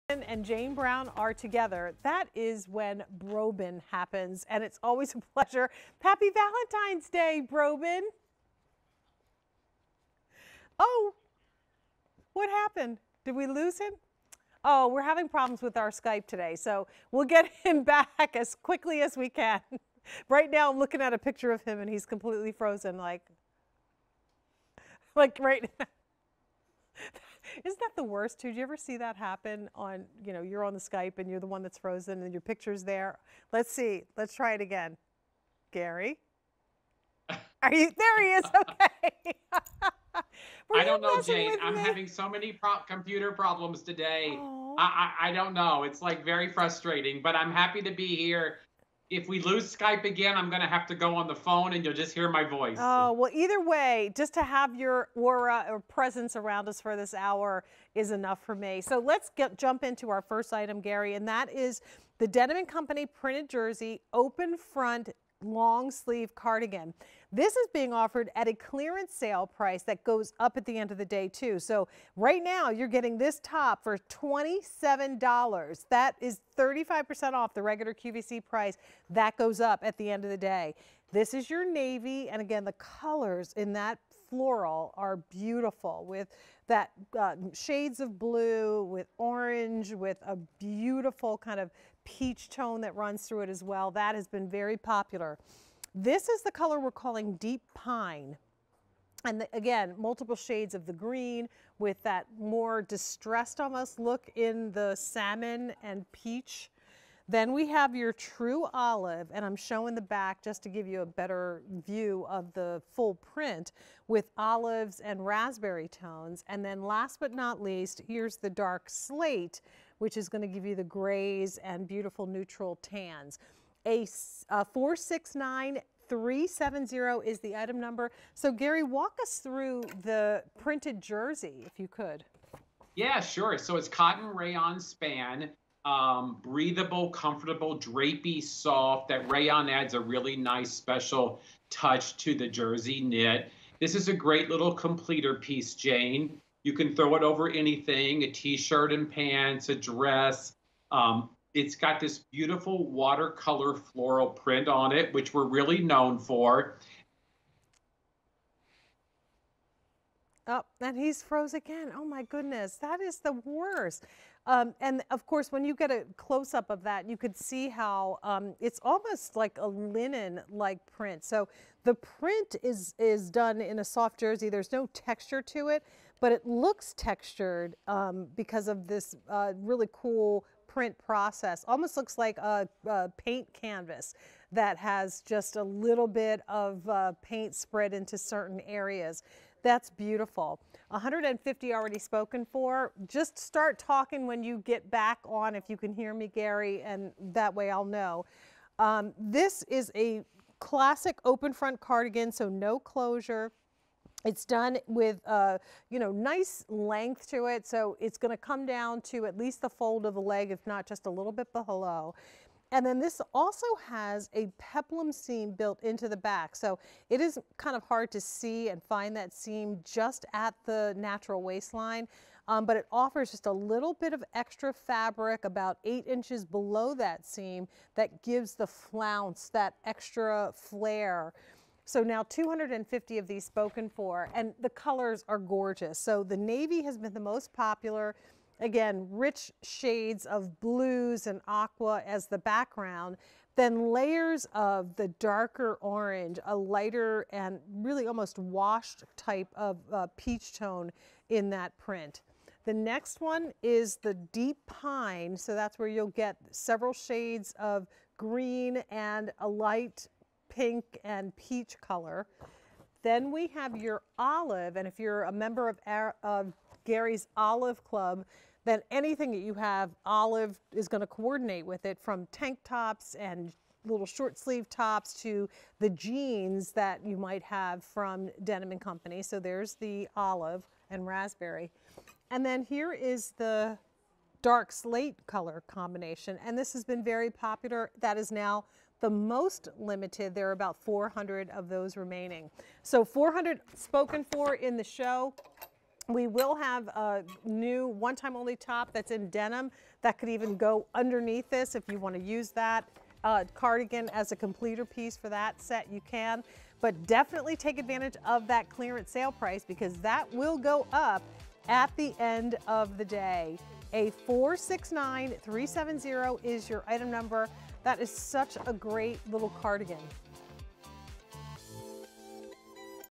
Denim & Co. Printed Jersey Open Front Long Sleeve Cardigan on QVC
Denim & Co. Printed Jersey Open Front Long Sleeve Cardigan

Match this flowing open-front cardigan with your favorite jeans and some white sneakers or flats to create a 'fit glowing with charisma. It's carefree, easygoing style at its best. From Denim & Co.&reg; Fashions.